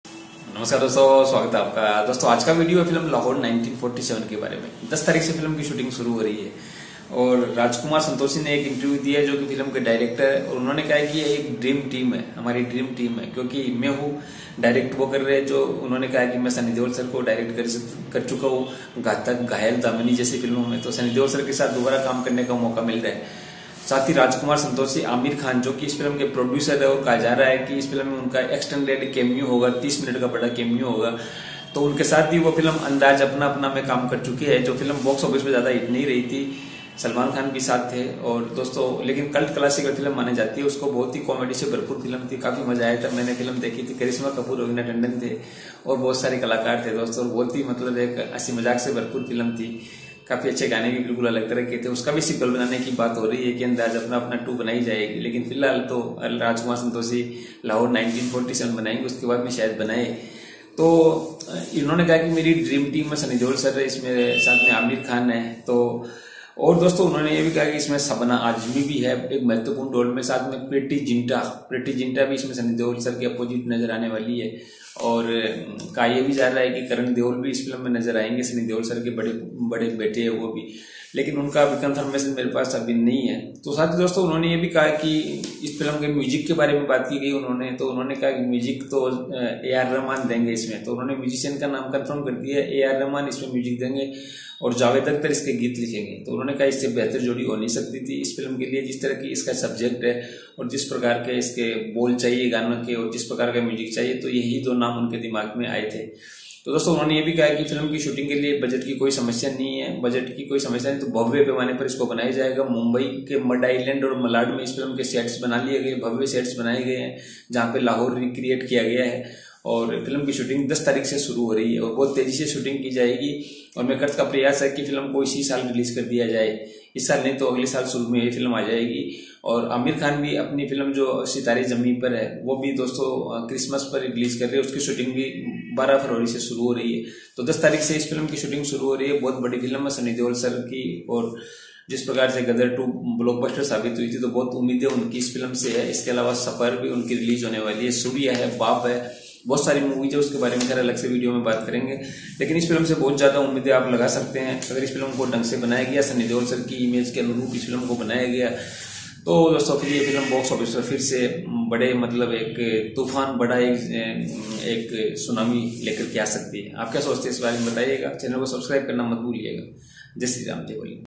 Lahore 1947 shooting starts 10th feb. rajkumar santoshi | sunny deol | aamir
Lahore 1947 latest update